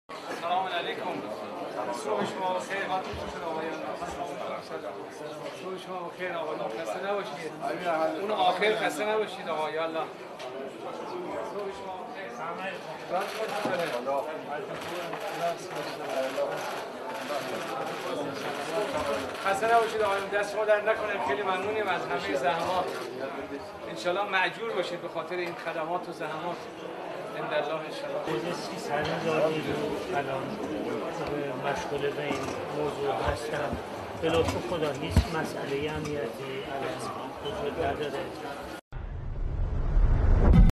President Ebrahim Raisi visited the election headquarters in the Interior Ministry to...
President Ebrahim Raisi visited the election headquarters in the Interior Ministry to review the process of holding the Iranian parliamentary and the Assembly of Experts elections.